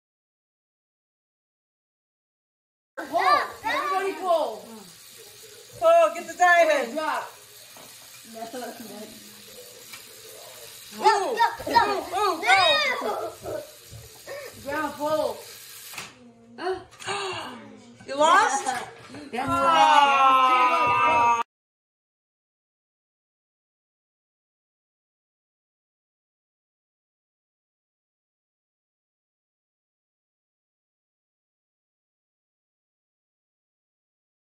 DIAMOND HEIST GAME
(Partner) Diamond Heist Game is a Game That Teaches Teamwork. Without it, you won't be able to steal the diamond and get away before the security car stops you.